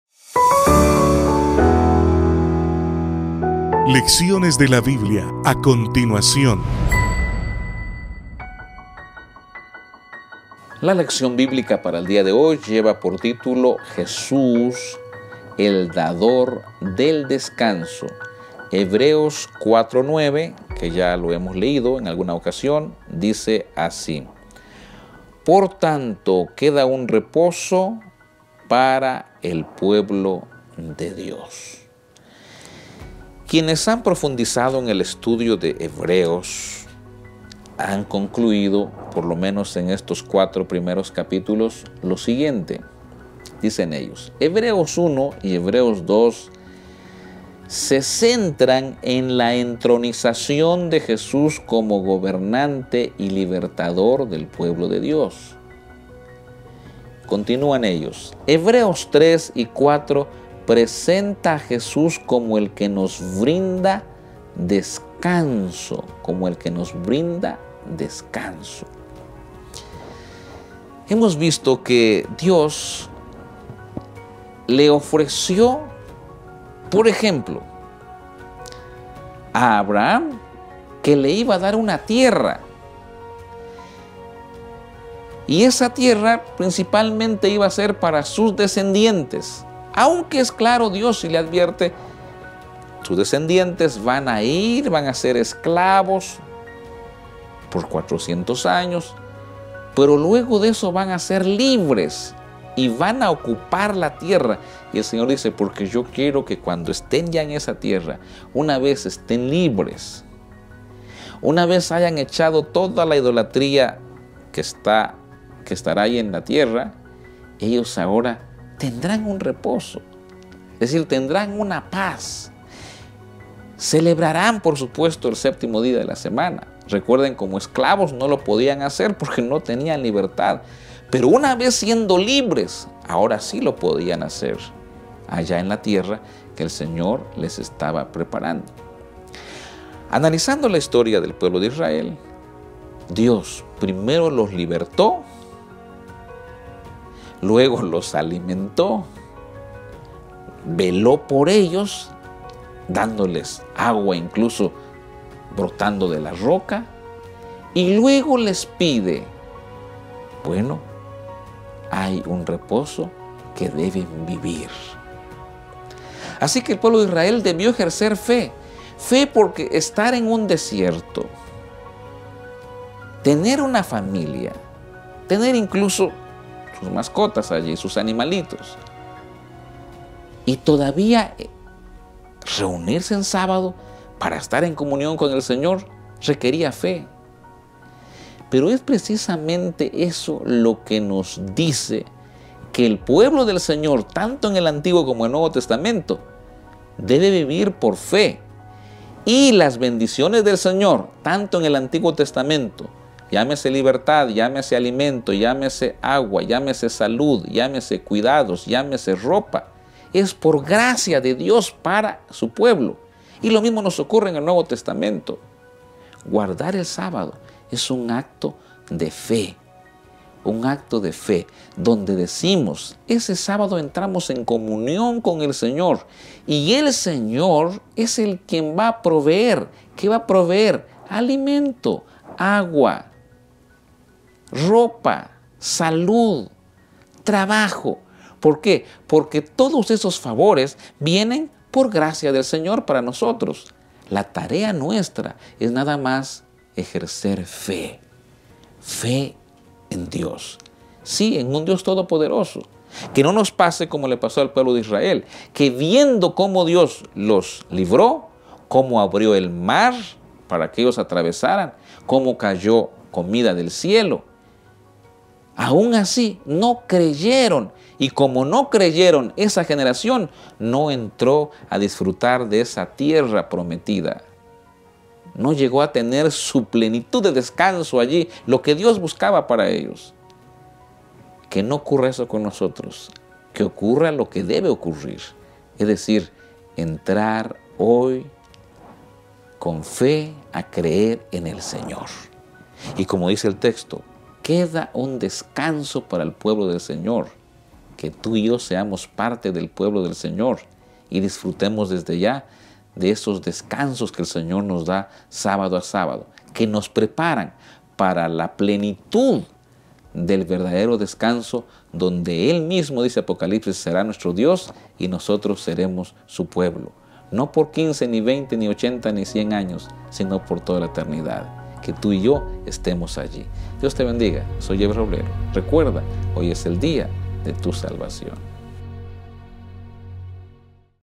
VIERNES 28 ENERO - LECCIONES DE LA BIBLIA
La lección de Escuela Sabática para este día se titula: JESÚS EL DADOR DEL DESCANSO, un tema presentado por el Pastor Ever Roblero.